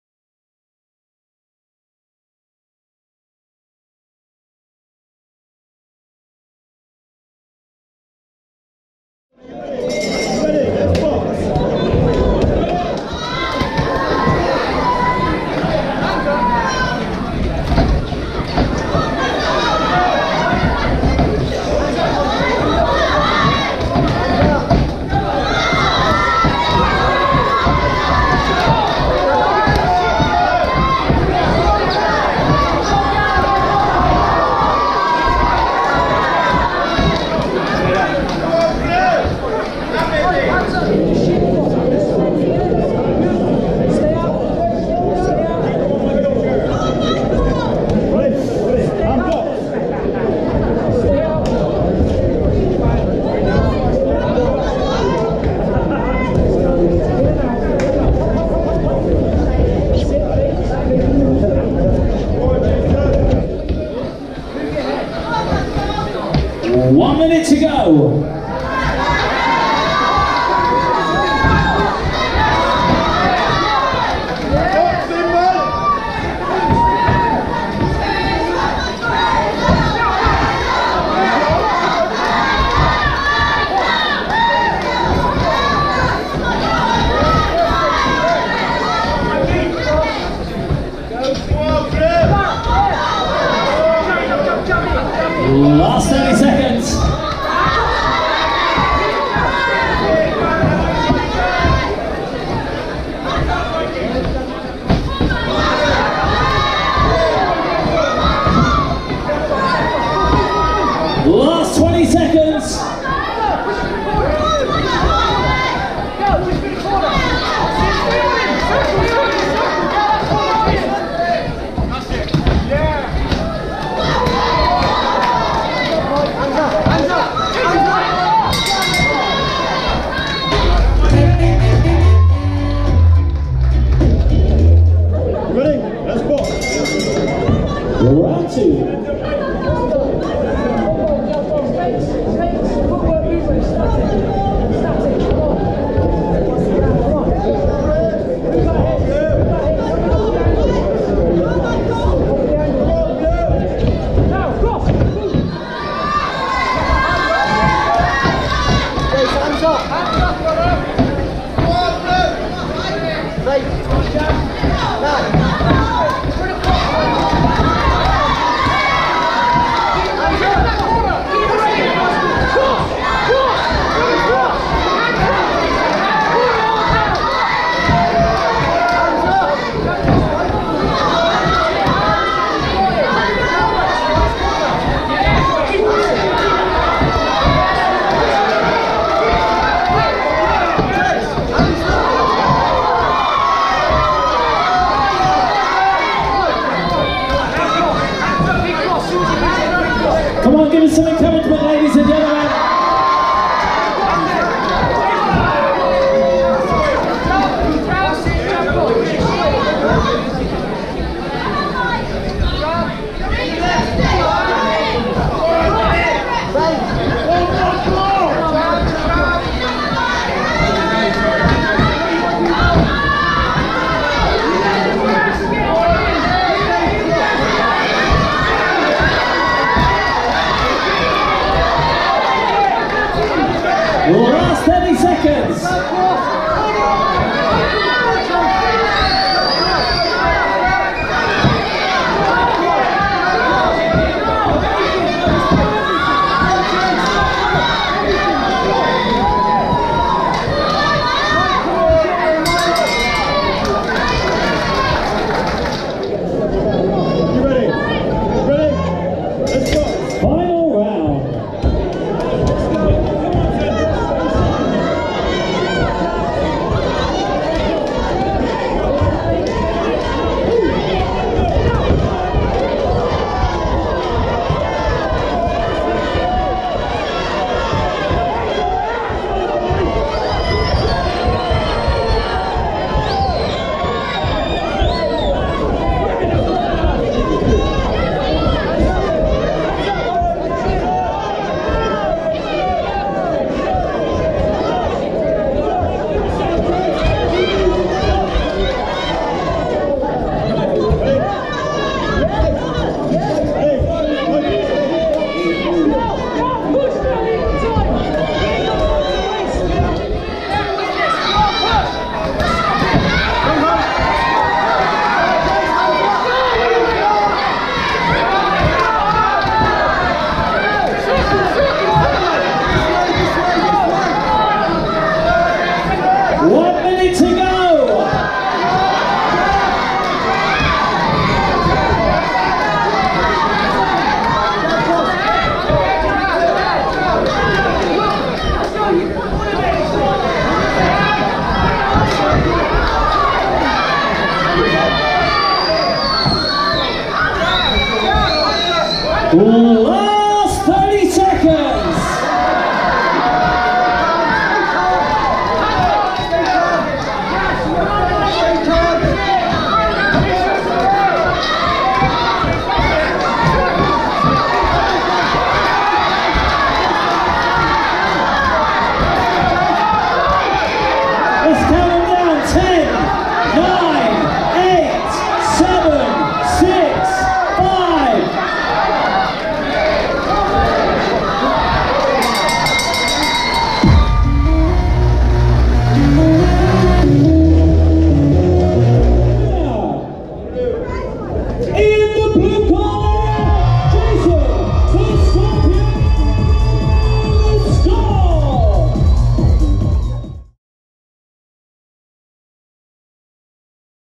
Ultra White Collar Boxing | Weston Super Mare | Michael Hamer VS Jason Scott
Weston Super Mare Ultra White Collar Boxing 8th April 2017

8 Weeks FREE training / Get in the best shape of your life/ Raise money for Cancer Research UK